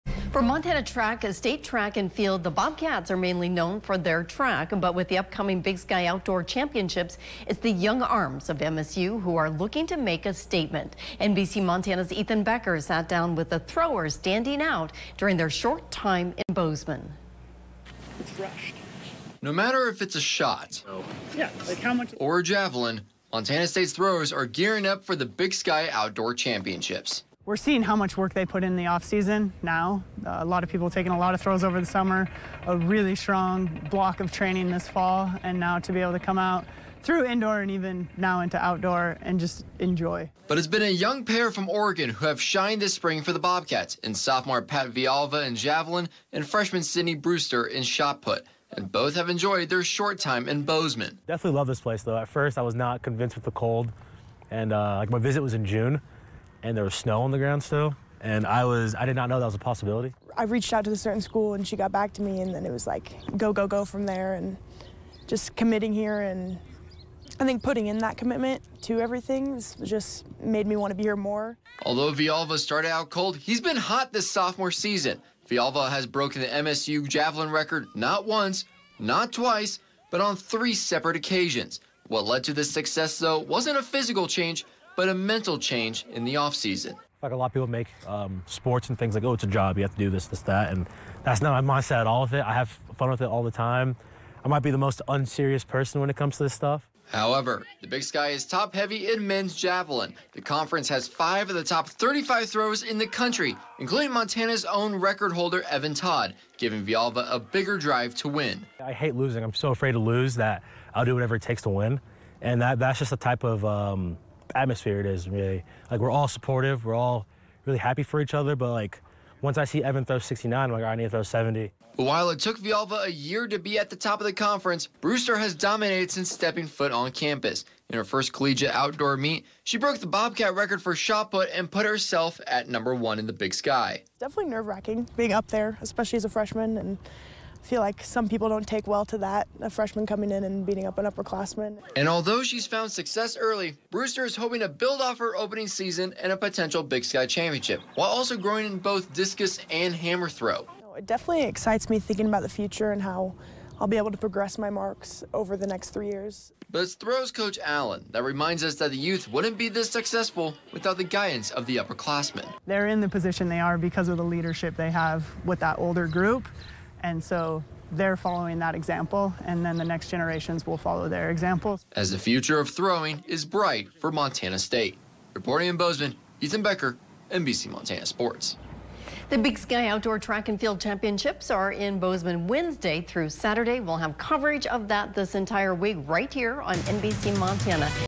MSU throwers look to continue success in Big Sky Track & Field Championships
Montana State University is well-known for its track and field team, primarily in track though compared to the field.

However, it's been the youth of the field team that has stepped up for the Bobcats this outdoor season. A pair from Oregon have shined in sophomore Pat Vialva for men's javelin and freshman Sydney Brewster in women's shot-put.